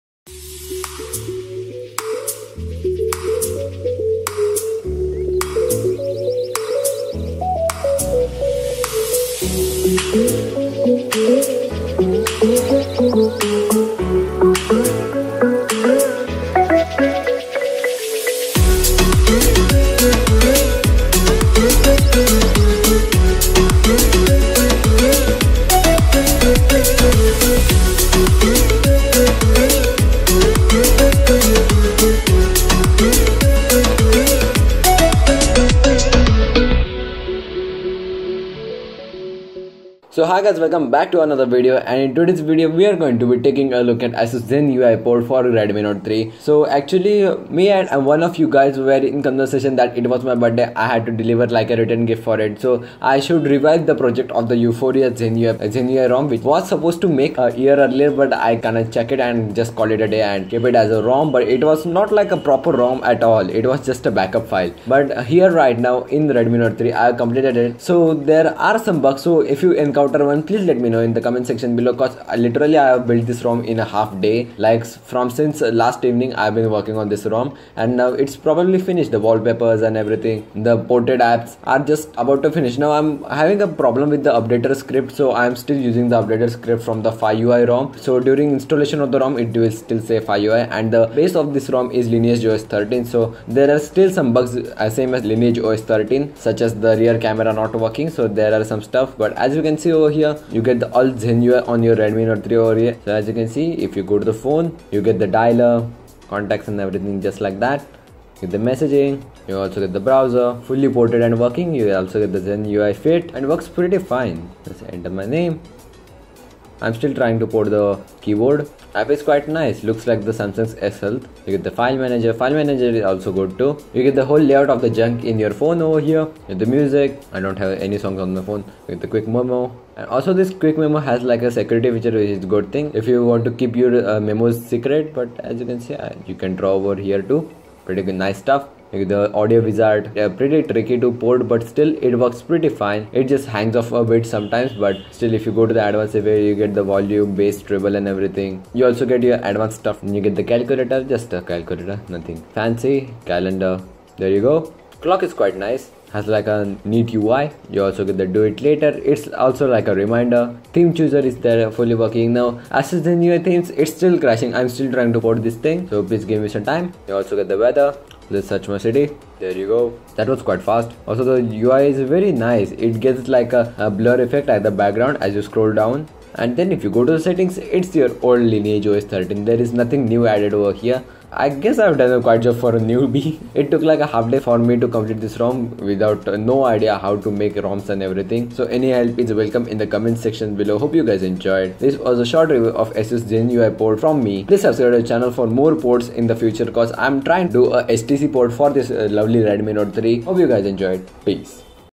Asus Zenfone (Zen UI 3.0) Port on Redmi Note 3
Thid my first ever rom hope you guys like it,It would be great if anyone would help me yo make a beautiful thread on xda and updater script.